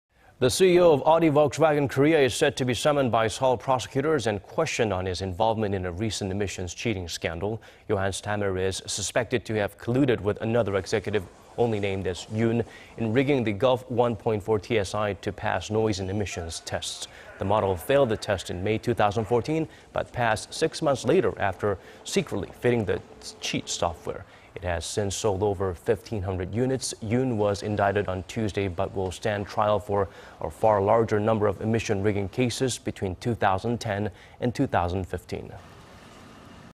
VW Korea CEO Johannes Tamer to be questioned by Seoul prosecutors on emissions cheating scandal
폭스바겐 총괄대표 사기혐의 곧 소환••• 배출가스조작 가담 정황
  The CEO of Audi Volkswagen Korea, is set to be summoned by Seoul prosecutors and questioned on his involvement in a recent emissions cheating scandal.
  Johannes Tamer is suspected to have colluded with another executive, only named as Yoon, in rigging the Golf 1-Point-4 TSI to pass noise and emissions tests.
The model failed the tests in May 2014, but passed six months later after secretly fitting the cheat software.
It has since sold over 15-hundred units.
Yoon was indicted on Tuesday but will stand trial for a far larger number of emission rigging cases between 2010 and 2015.